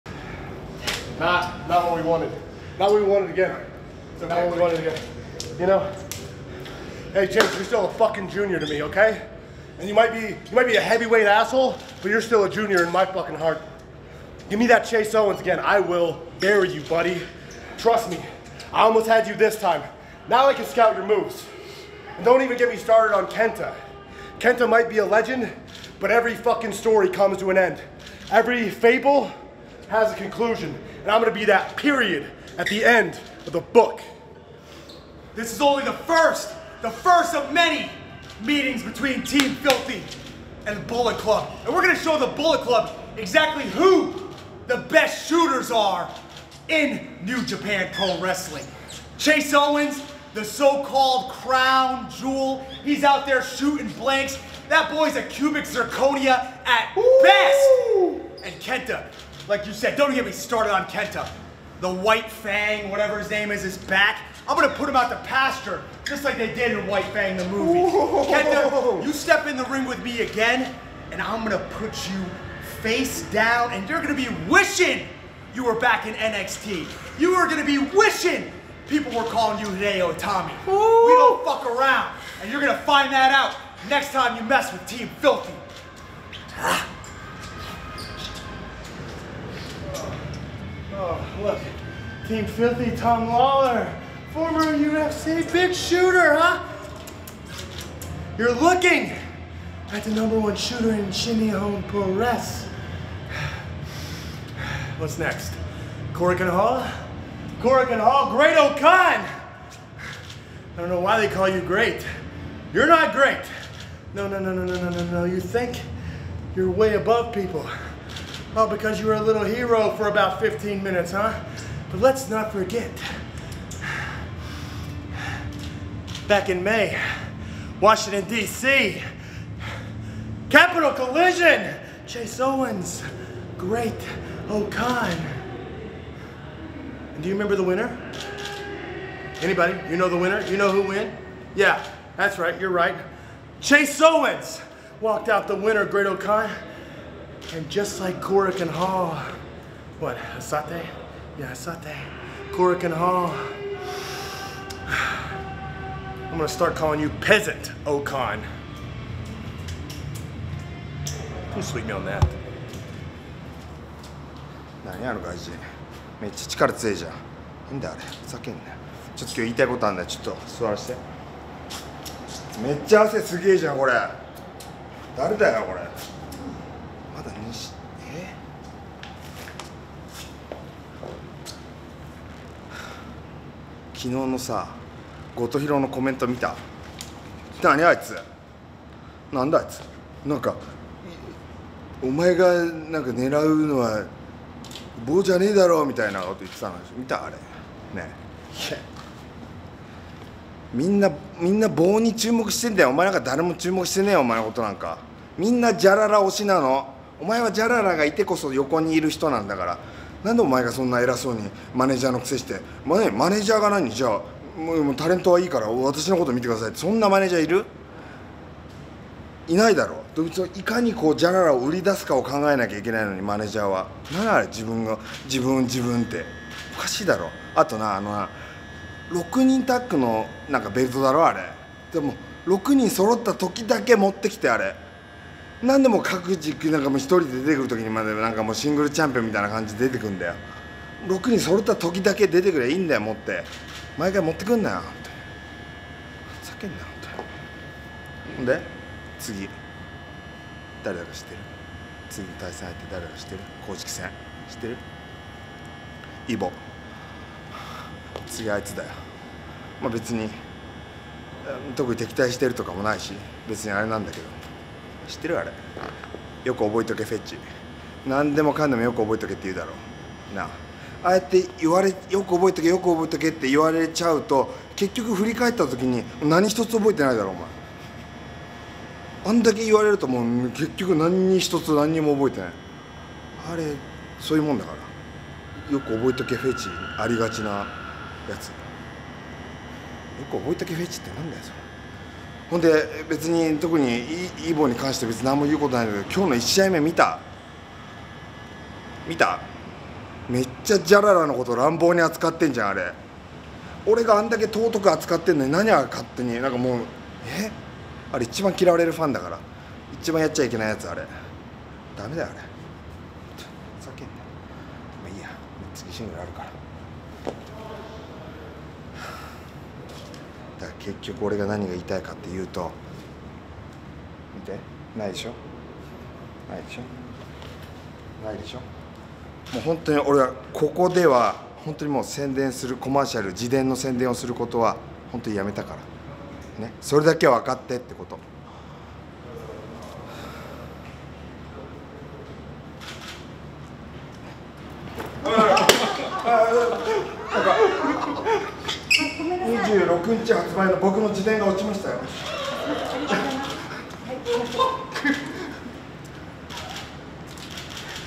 KENTA「べつに、特に敵対してるとかもないし、あれなんだけど、知ってるアレ『よく覚えとけ』フェチ」7.24 G1CLIMAX32 Backstage comments: 2nd match
2022年7月24日(日)G1 CLIMAX 32
東京・大田区総合体育館
第2試合：
ロイス・アイザックス & トム・ローラー 🆚 チェーズ・オーエンズ & KENTA

July 24, 2022 “G1 CLIMAX 32 Day5”
Tokyo・Ota City General Gymnasium
2nd match:
Royce Isaacs and Tom Lawlor 🆚 Chase Owens and KENTA